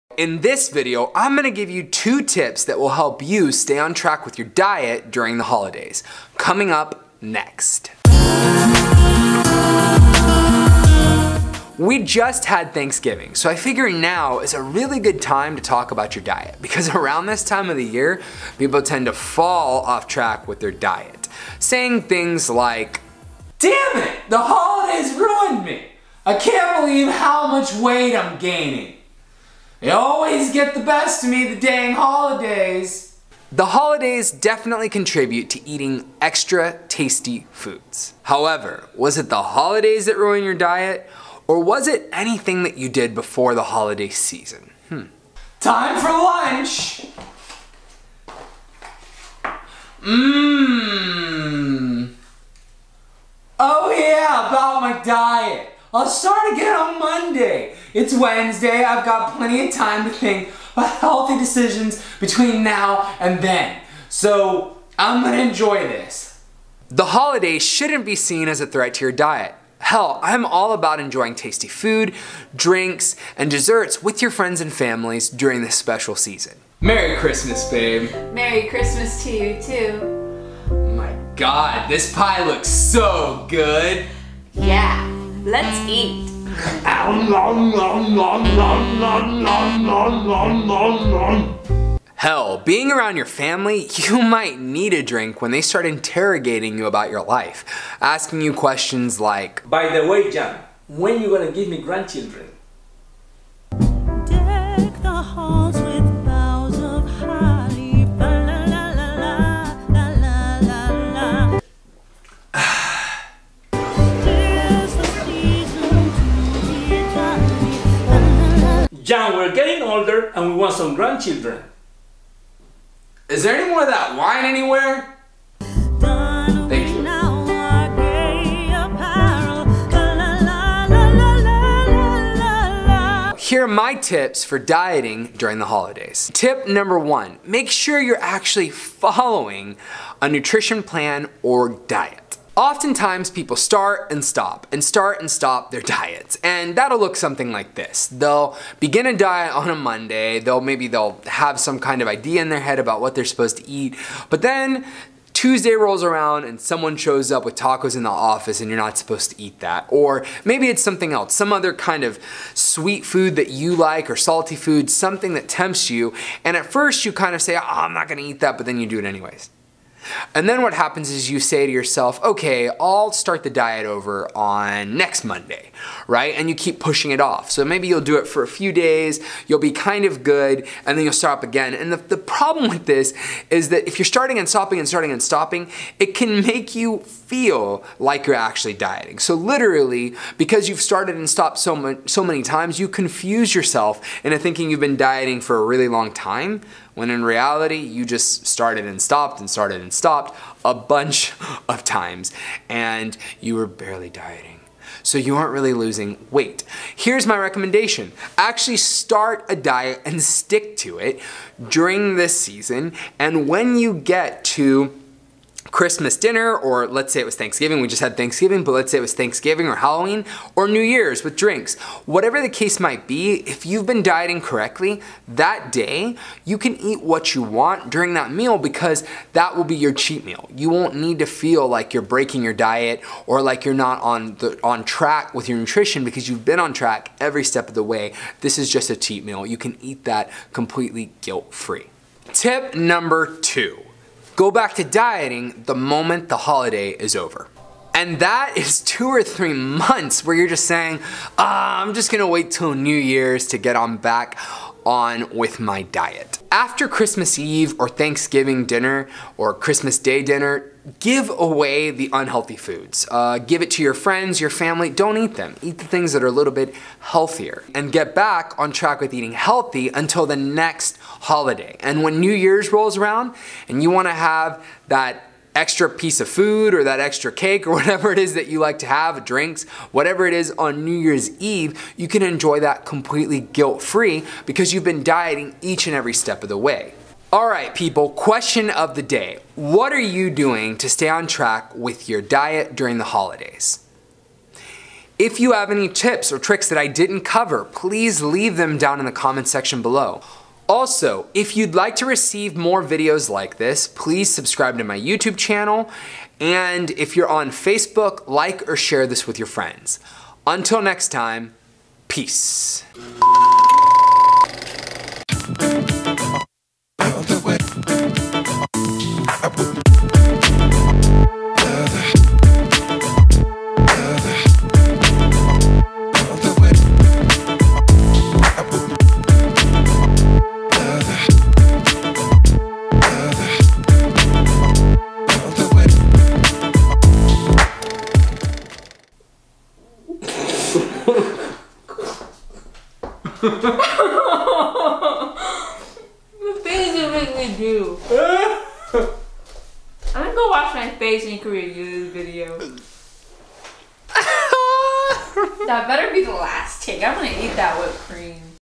How to stay on track with your DIET during the HOLIDAYS | 2 Tips for dieting during the holidays
Dieting during the Holidays can be tough! Here are some tips to help you stay on track with your diet during the holidays while still enjoying some tasty foods along the way!

I'm talking from the heart here lol! I remember that when I was struggling with my weight I would always drop the ball during the holiday season.

I'm hoping that the information will save you from the mistakes I made in the past.

In the video I give 2 tips that I think will Help you.

1. Make sure you are following a diet. Now, what do I mean by that?  people start diets and stop them start stop them over and over again. Because Maybe “It's Just because you keep eating unhealthy foods that people bring to work and offer you” “Maybe you eat your kid’s food” “You succumb to peer pressure when, Sherryl and the girls want you to drink! “It’s Just one, More drink. It’s Just one drink.
SO, Stick to your diet and until the actual holiday. When that Christmas meal and drink times around comes around you can eat guild free because this one cheat meal is going ruin your diet!

2. Go back to dieting the moment the holiday is over!
I say this because people will use the entire holiday season as an excuse to drop off with their diet. That  is 2-3 months where you are saying ahh I’m just gonna wait till after new years to get on track with my diet! Here’s is the thing, you can gain a lot of weight in 2-3 months. Seriously, you can also undo you worked so hard to lose by just dropping your diet for 3 months! So, after Thanksgiving Or Christmas dinner don’t eat all the left over unhealthy foods! Give them away to your friends or family! And start eating healthy until the next holiday! And when new years rolls around and you want to eat a big meal and enjoy some Champagne just go right ahead! Enjoy the food and drink guilt free because you earned it!

Those are just 2 tips to help you stay on track with your diet during the Holiday.

FUN FACT: My Dad Just Gonzalez Is in this episode. He taught me me how to speak in front of audiences and how to use my personal experiences as stories to help people. So, I was thrilled to have him in this episode.